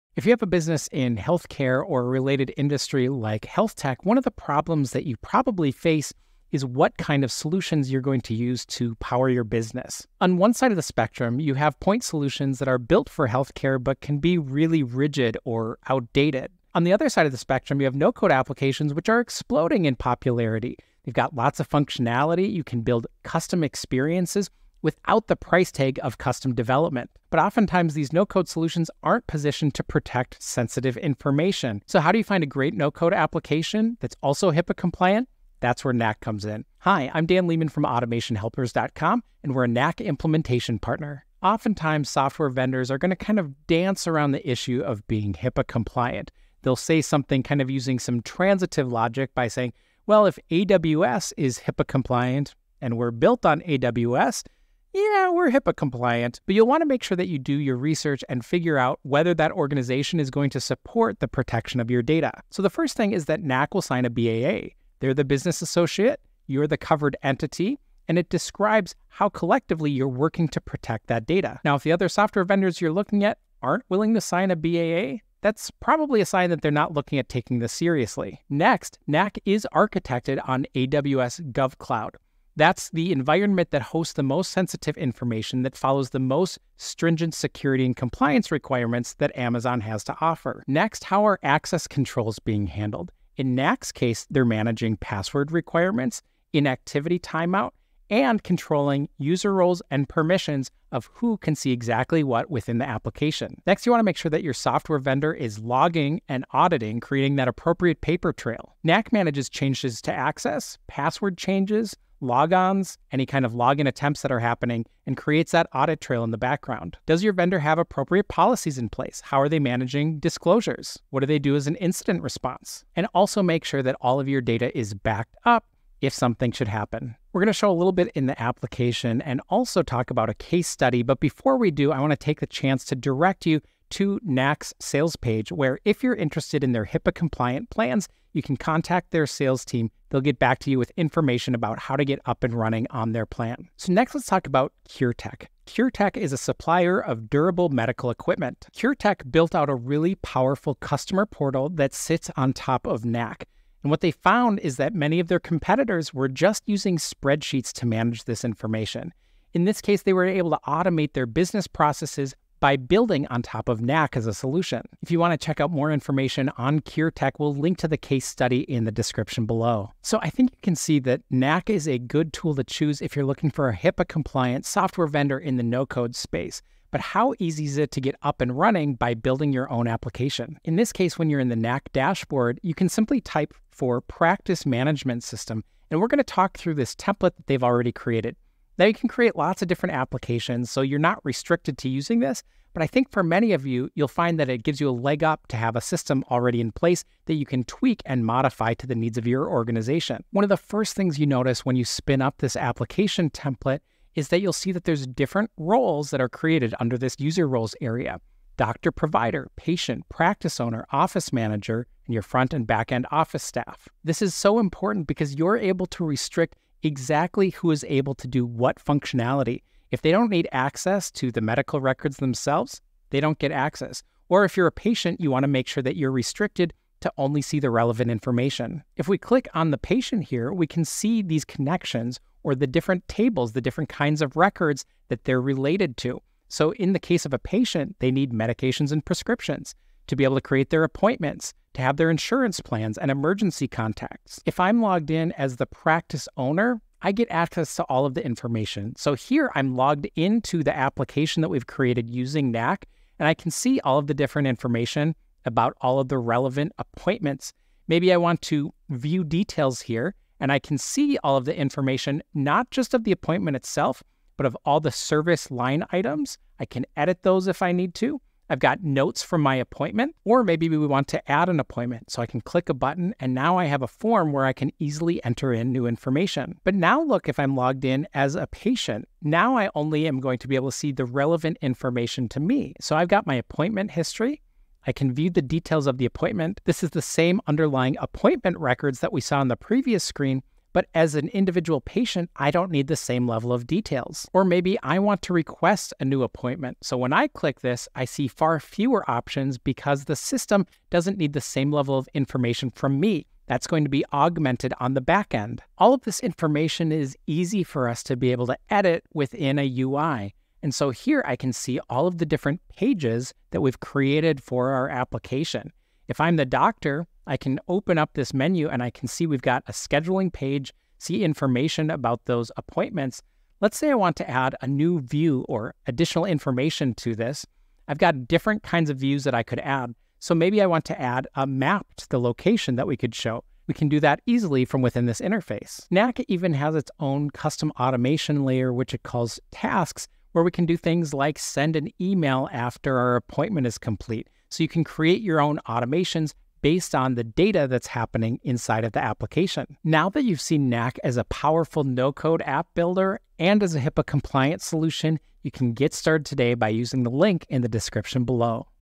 Build HIPAA-Compliant Apps without Code
🏥 Welcome to the world of HIPAA compliance and no-code app development with Knack! Finding the right solution to power your business can be challenging if you're in the healthcare or health tech industry.

💡 In this video, we'll explore the crucial need for HIPAA compliance in healthcare software and how Knack offers a no-code solution that protects sensitive patient data.

🔐 Discover the key features that make Knack HIPAA compliant, including business associate agreements, robust access controls, logging and auditing, incident response, and data backup.

👩‍⚕️ Learn how Knack empowers healthcare professionals to build customized applications without the need for extensive coding knowledge, enabling automation and efficient business processes.

📈 Explore a real-life case study of CureTech, a durable medical equipment supplier, leveraging Knack to streamline their operations and deliver an enhanced customer experience.

🔗 Ready to explore the possibilities of Knack for your healthcare business?